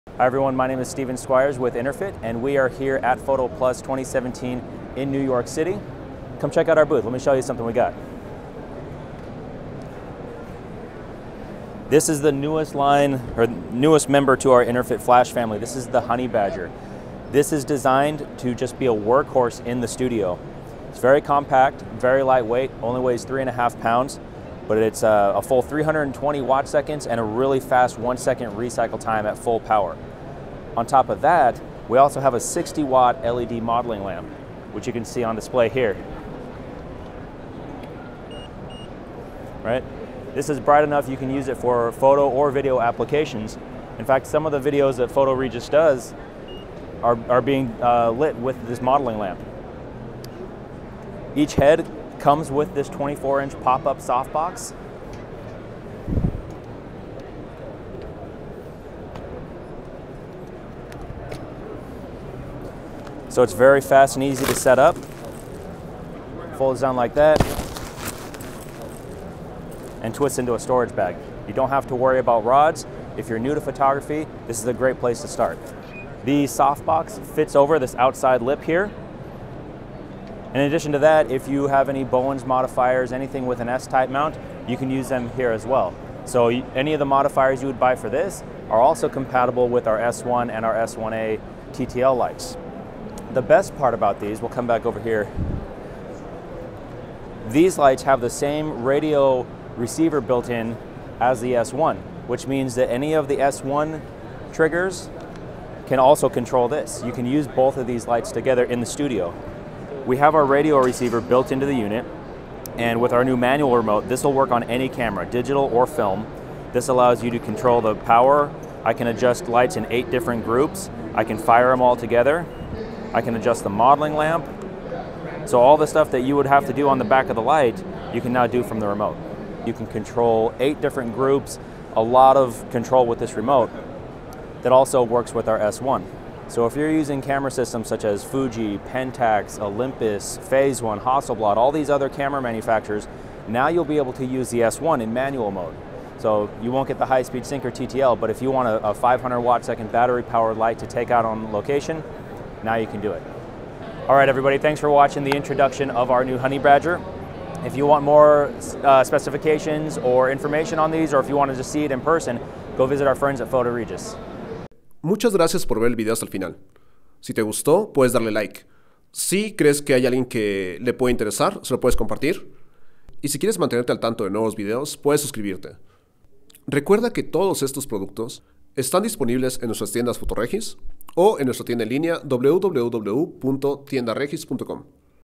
Vistazo del Honey Badger, un híbrido de flash y luz continua. Compacto, económico y versátil.
Acompaña a Steven Squires Gerente de Producto de Interfit mientras nos muestra las principales características del pequeño y feroz flash de estudio Honey Badger. Desde Photoplus NYC, 2017.

0:11 Honey Badger
0:36 Lampara de modelado
0:51 Accesorios
1:37 Sistema de radio